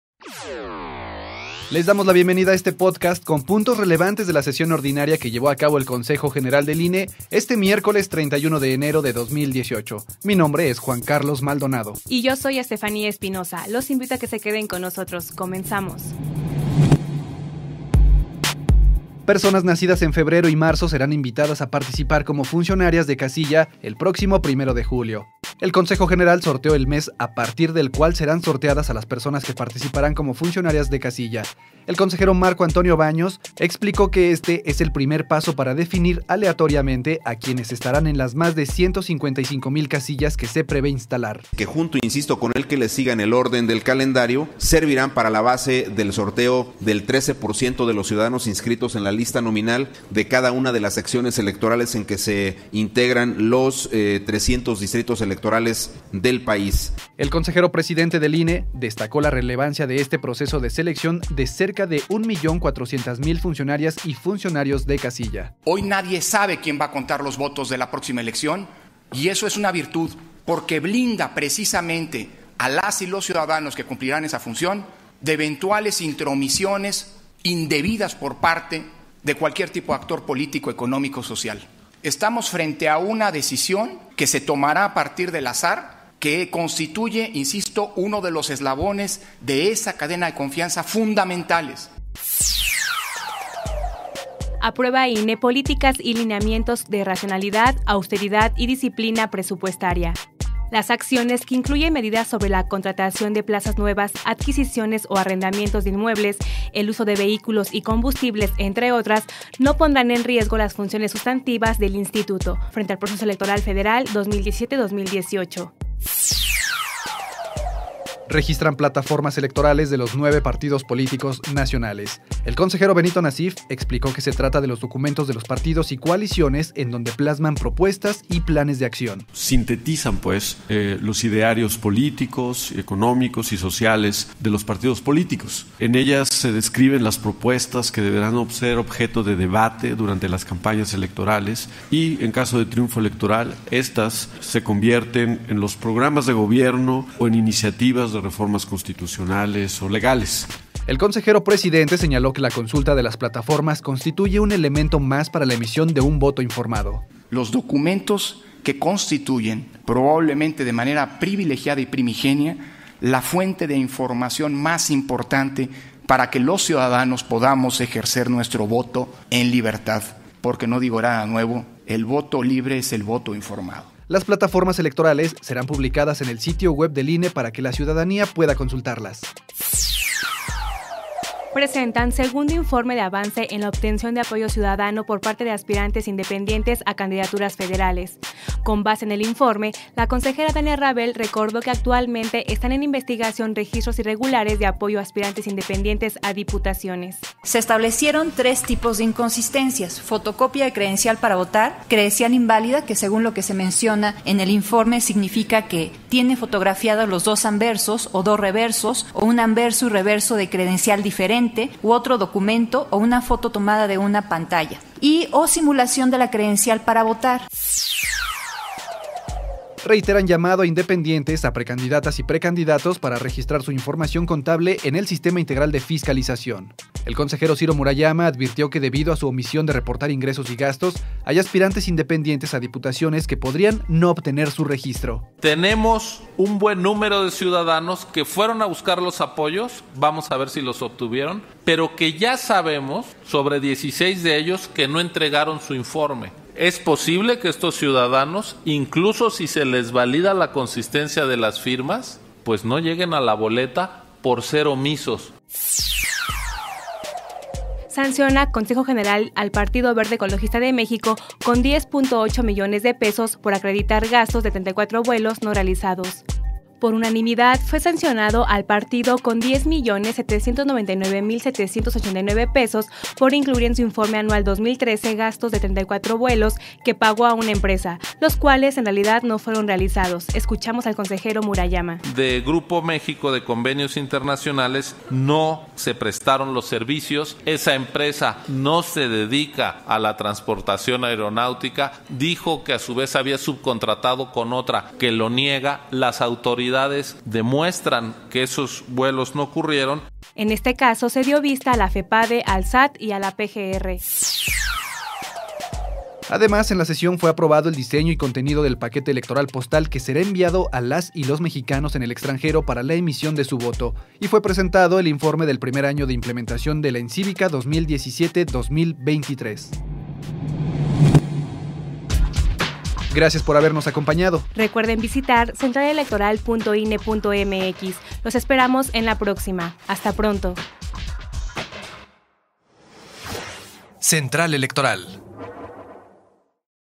Ciudadanos/as nacidos en febrero y marzo podrán ser funcionarios de casilla
El Consejo General sorteó el mes de febrero, que junto con marzo, será tomado como base para sortear a las personas que participarán como funcionarios de casilla el próximo 1 de julio; además registró las plataformas electorales de los nueve Partidos Políticos Nacionales.